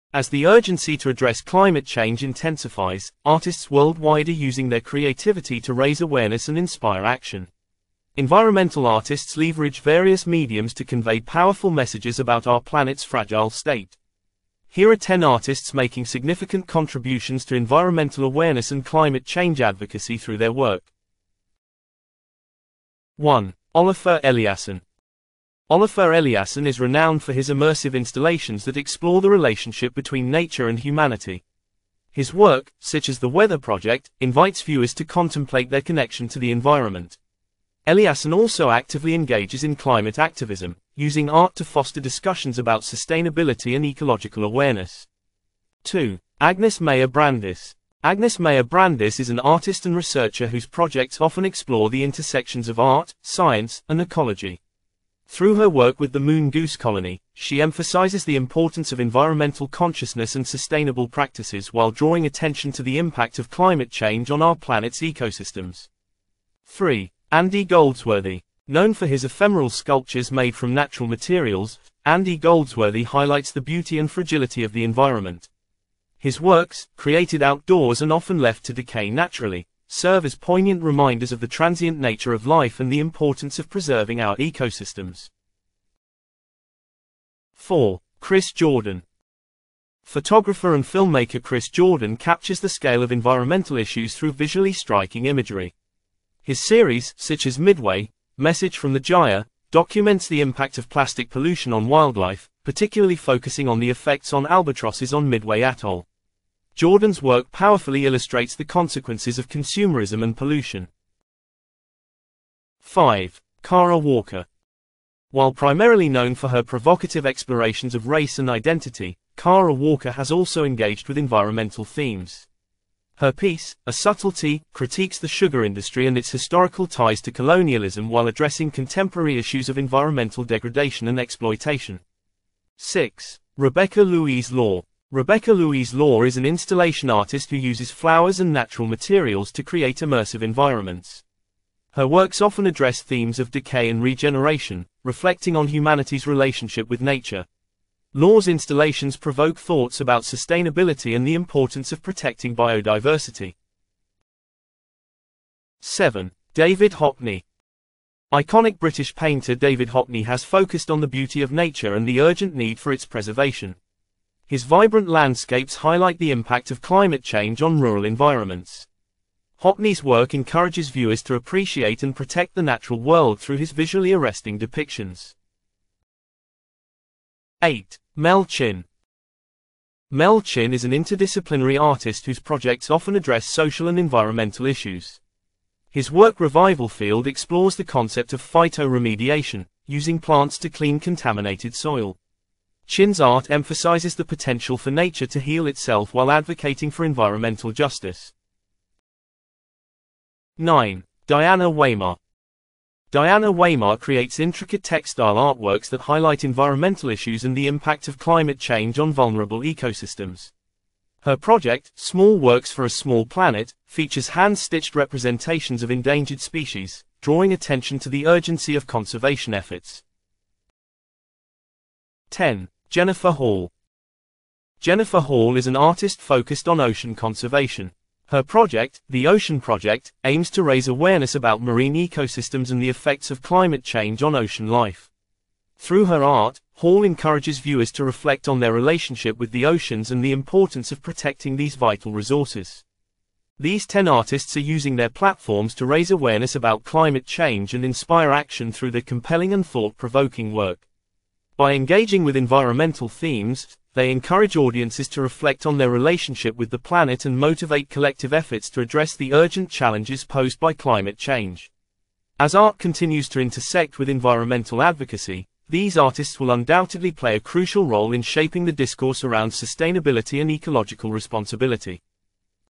Top 10 Environmental Artists Drawing Attention to Climate Change
Meet the top 10 environmental artists whose impactful works are bringing climate change issues to the forefront of public consciousness.